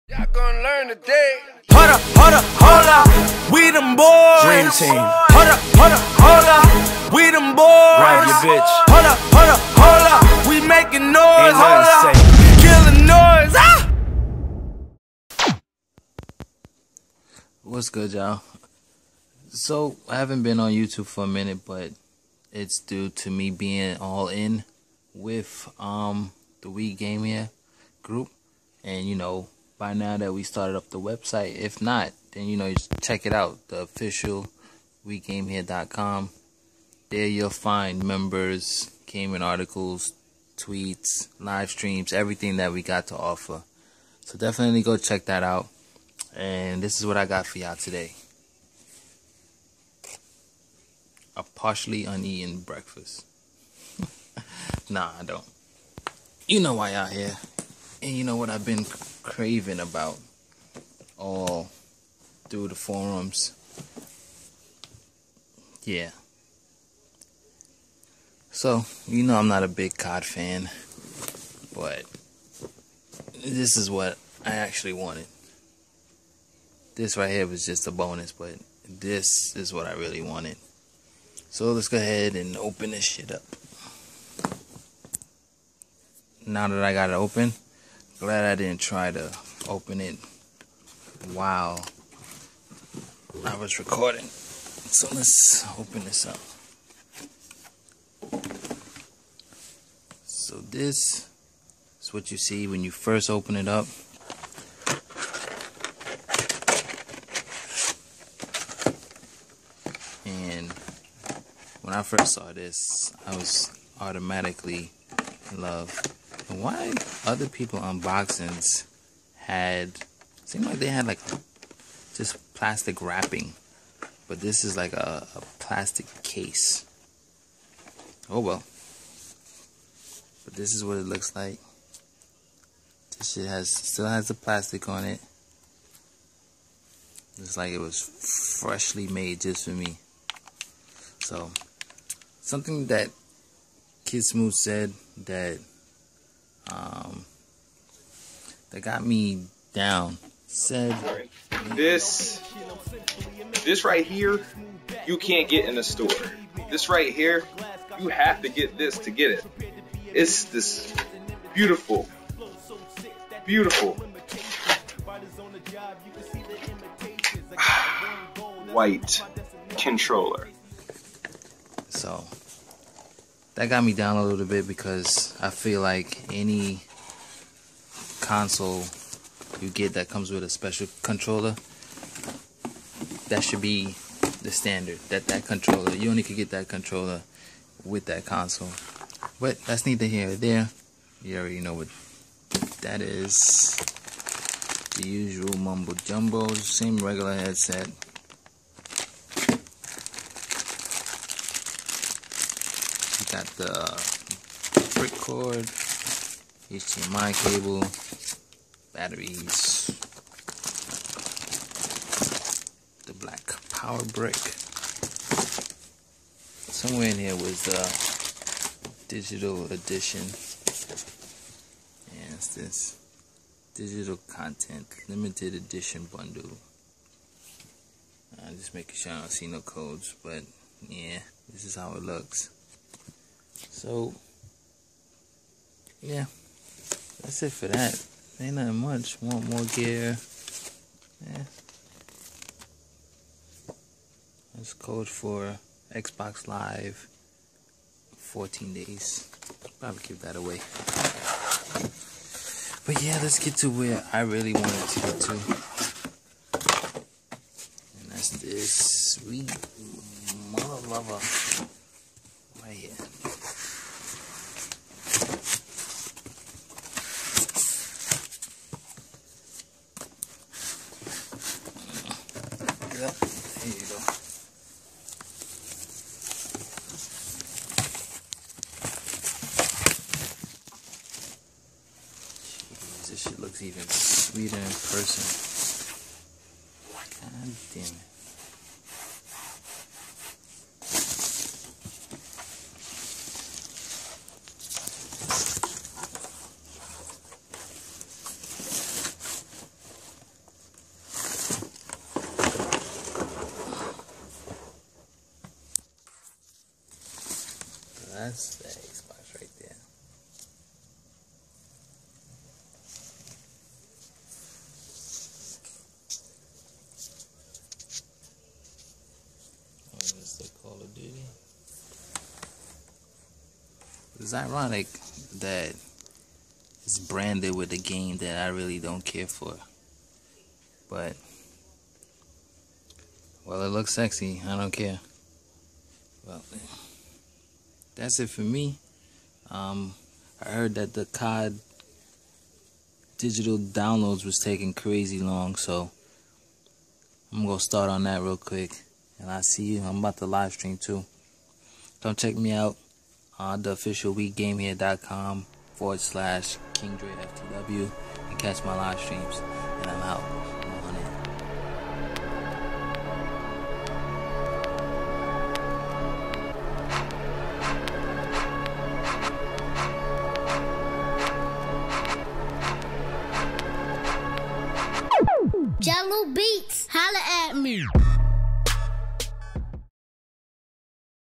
My Bundle Of Joy aka COD Unboxing
Watsup i traded in my xbox about a week ago when i heard they was dropping prices on the bundles so i didnt want to lose that value. The only game i didnt get to play was Sunset Overdrive which i can play later on but anyway hope you enjoyed the video.